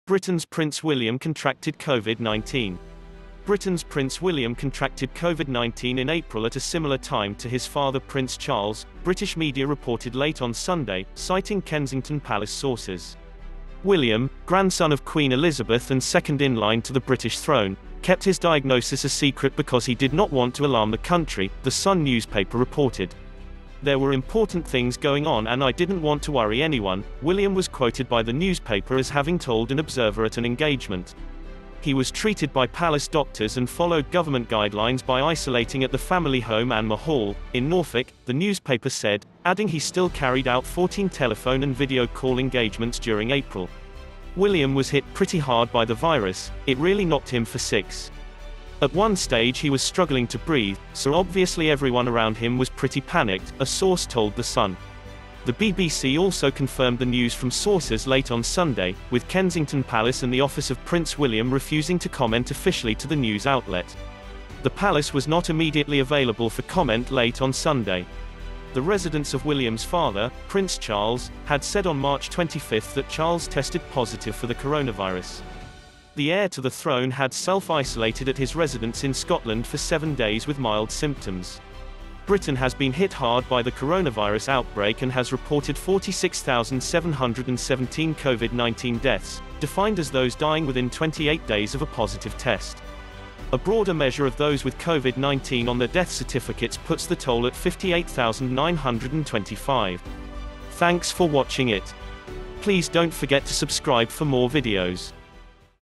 Britain's Prince William contracted COVID-19
Britain's Prince William contracted COVID-19 in April at a similar time to his father Prince Charles, British media reported late on Sunday, citing Kensington Palace sources. William, the grandson of Queen Elizabeth and second-in-line to the British throne, kept his diagnosis a secret because he did not want to alarm the country, The Sun newspaper reported.